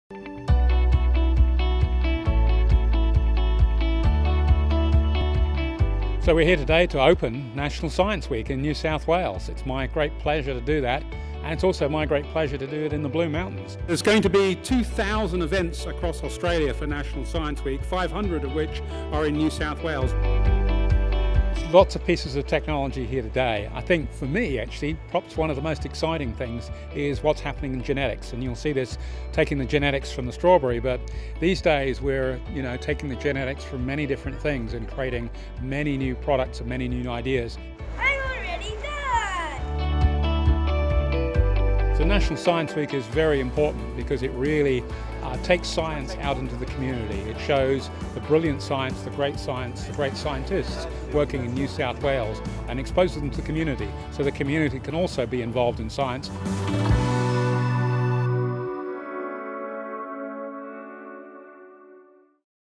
National Science Week launches in the Blue Mountains
This year's National Science Week was launched in the Blue Mountains by NSW Chief Scientist and Engineer, Hugh Durrant-Whyte. The annual celebration of all things science and technology includes nearly 500 events around the state designed to inspire and educate the community.